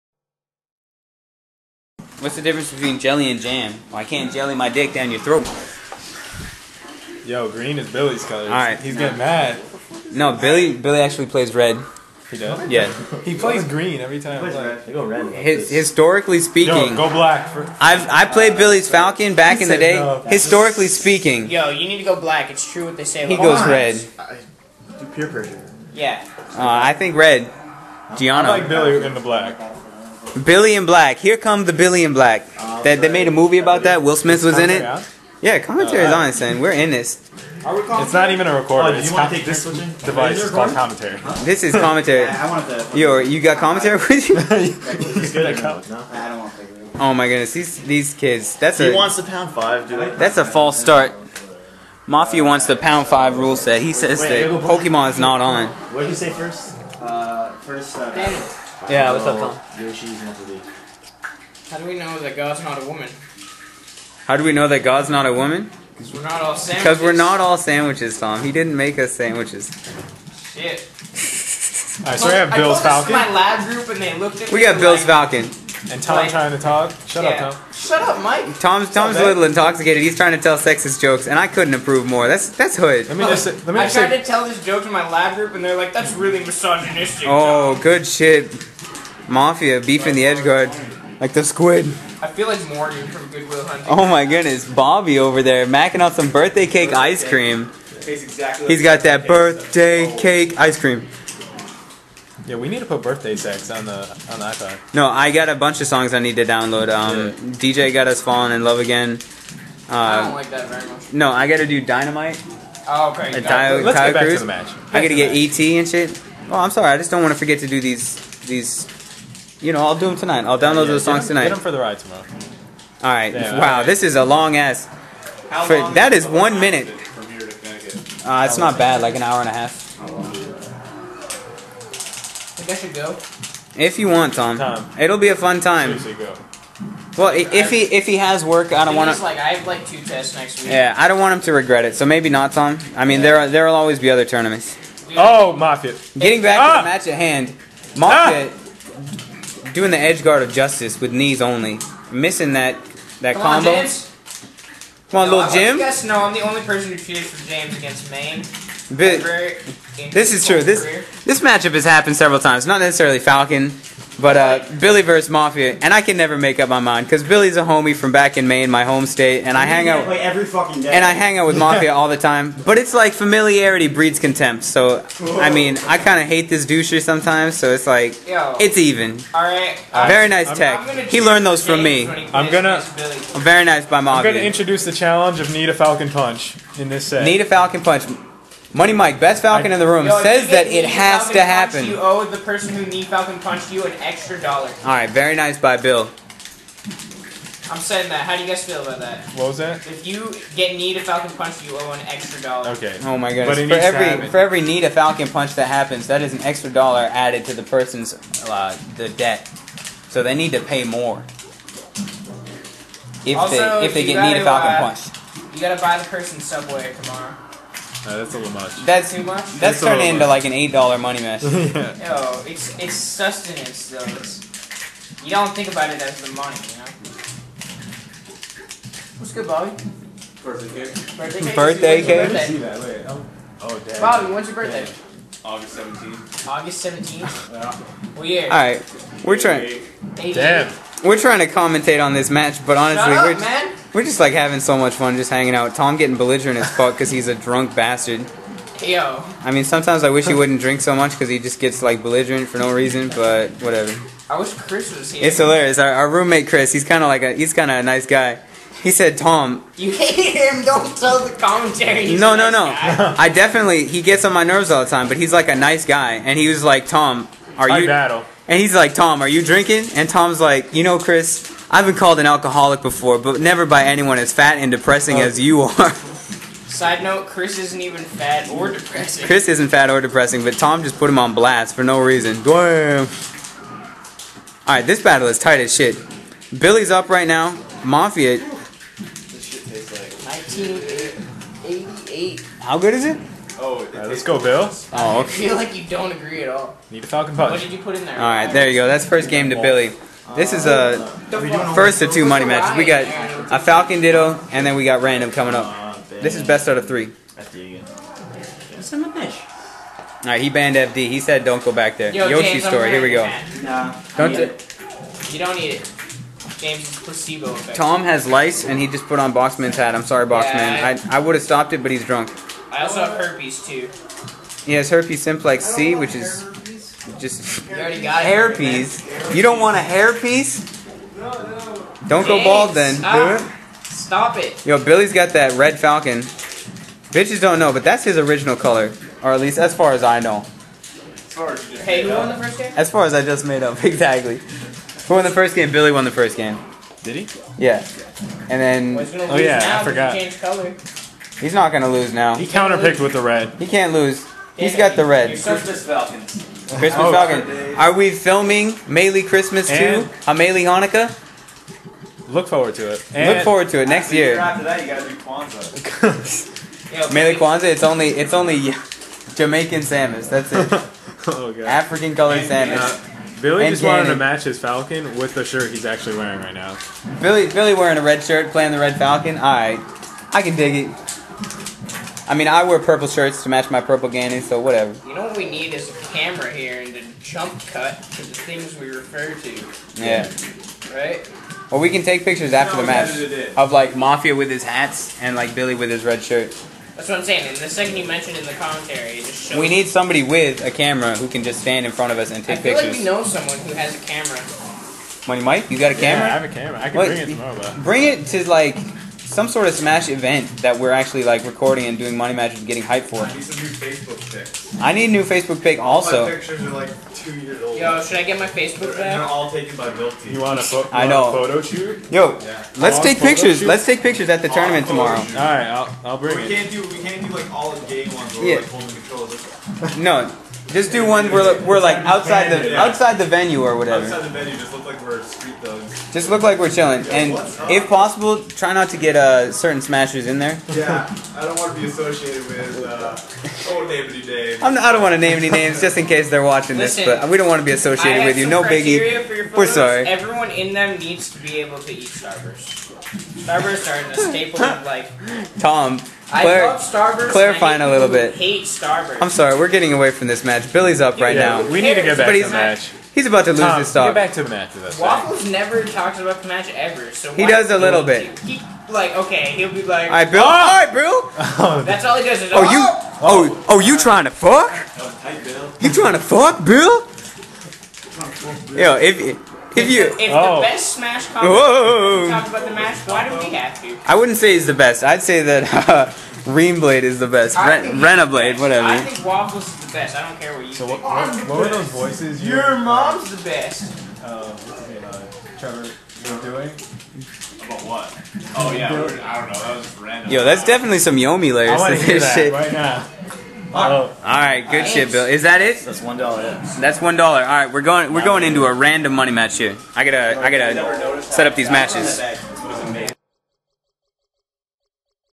Th0rn (Black) vs. Mafia (Green) MM
MM from a pre-tourney smashfest in Boston, MA